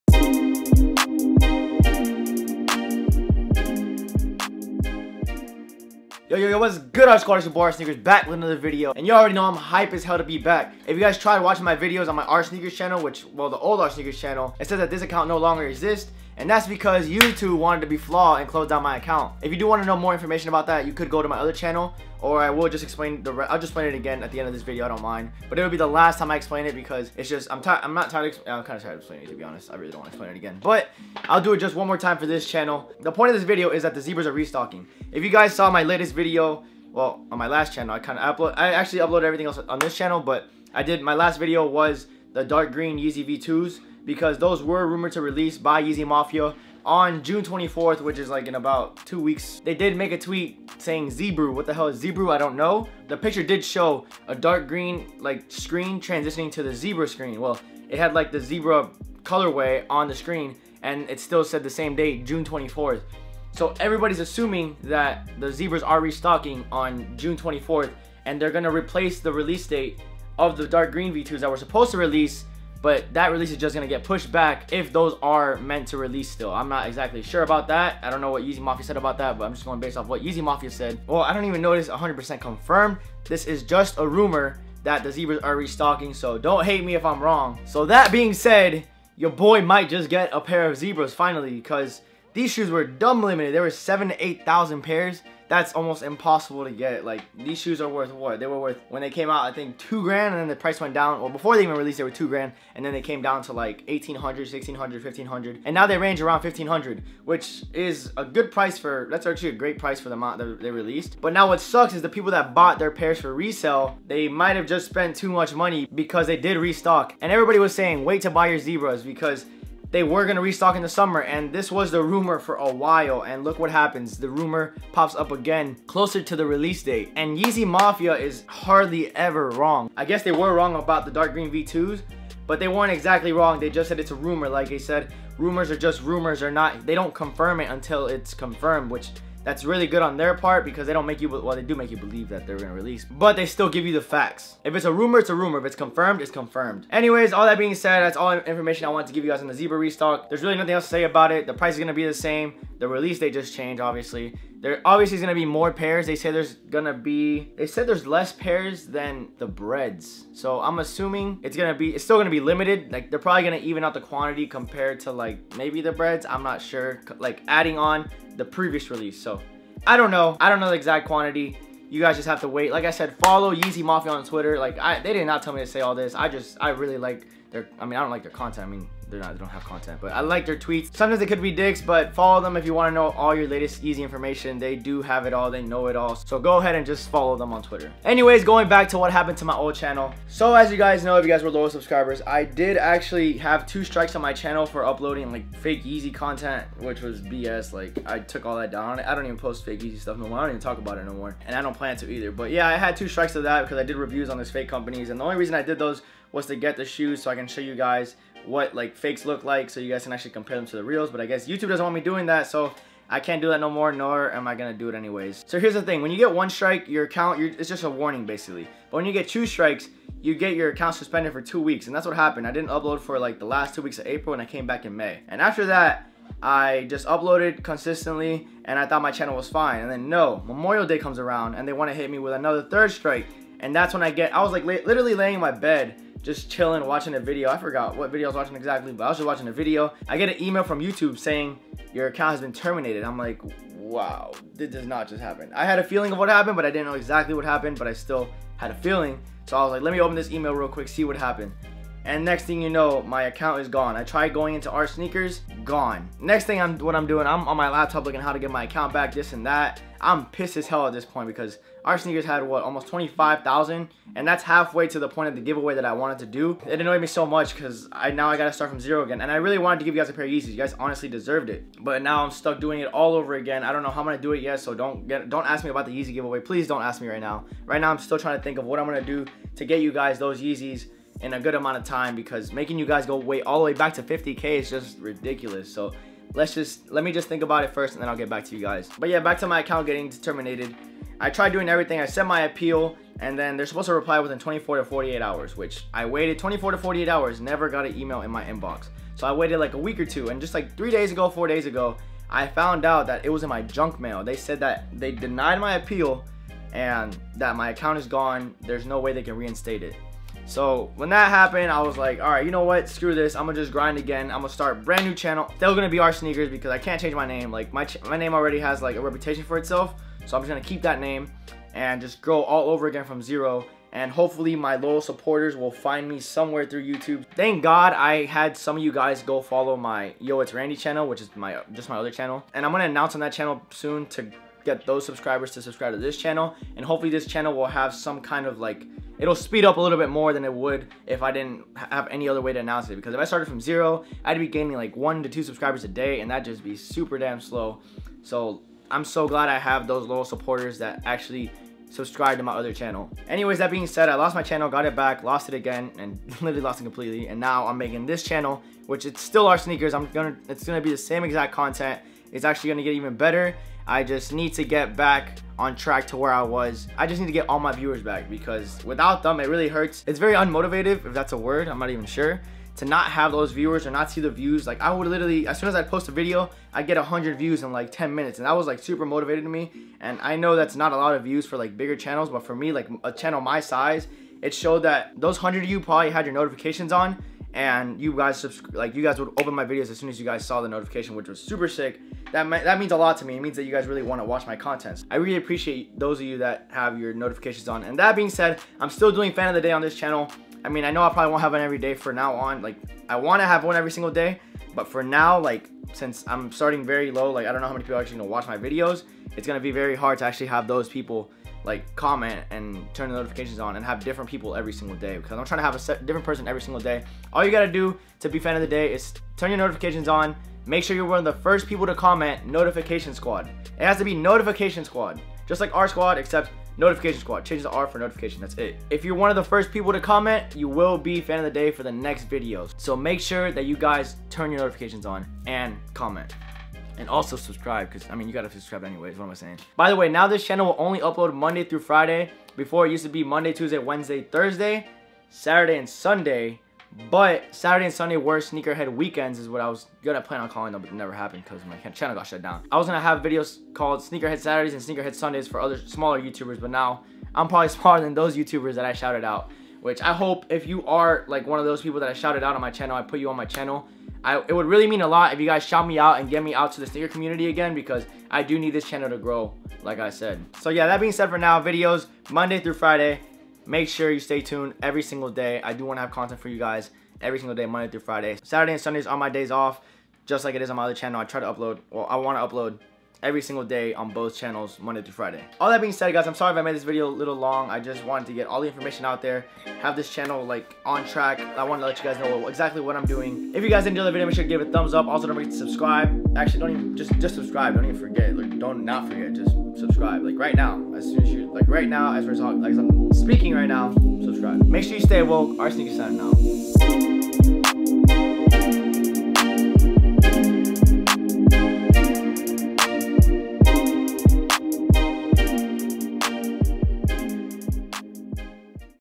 YEEZY V2 ZEBRA RESTOCK
I had to talk about the zebra restock and also how this channel is gonna come up ;) R sneakers is back!

-MY LINKS-
MY KNIFE